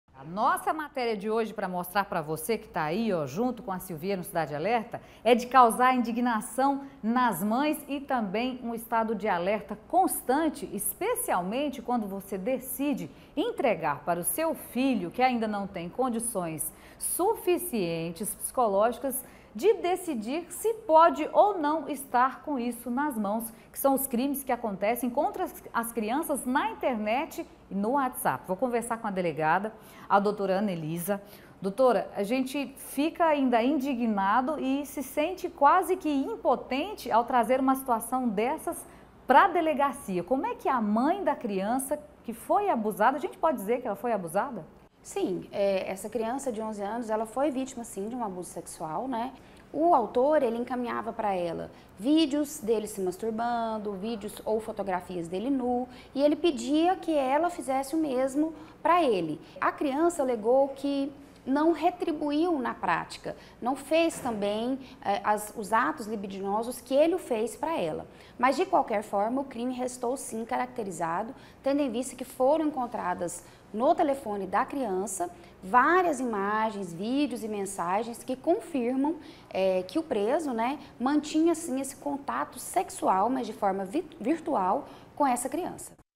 PRESO SUSPEITO DE ALICIAR MENINA DE 11 ANOS
Preso um homem suspeito de aliciar uma menor de onze anos pelo celular.